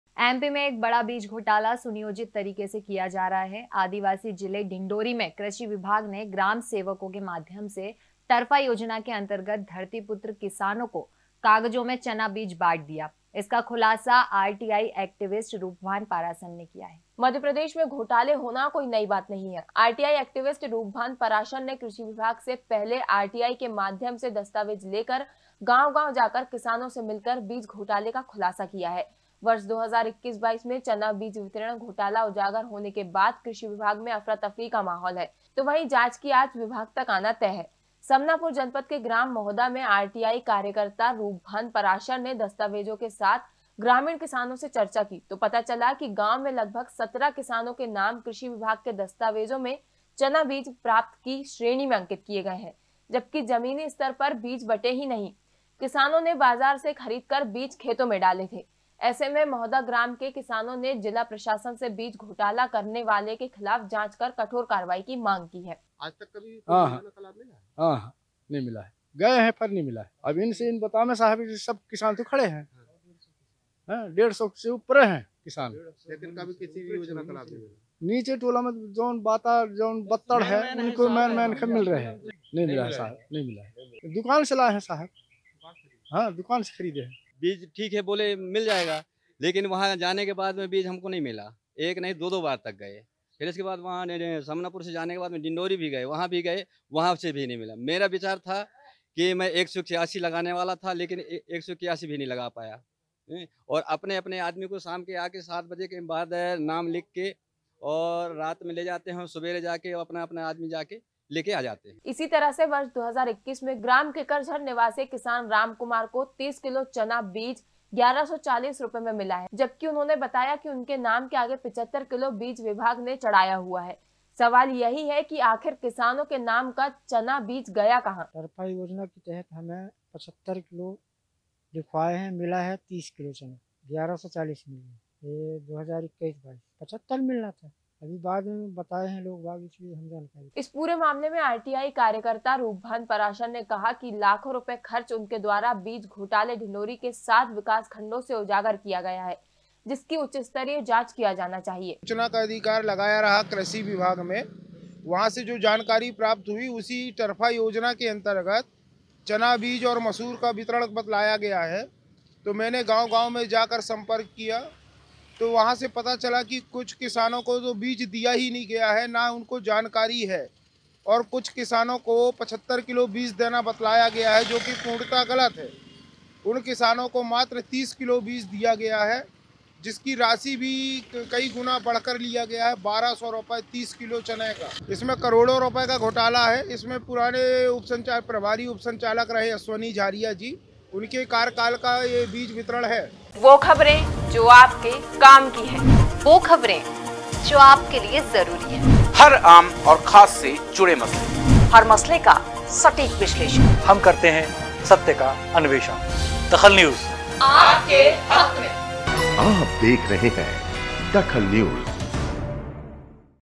DAKHAL NEWS कृषि विभाग ने कागजों में बाट दिए बीज
कृषि विभाग ने कागजों में बाट दिए बीज
अफसरों ने किया करोड़ो रु का बीज घोटाला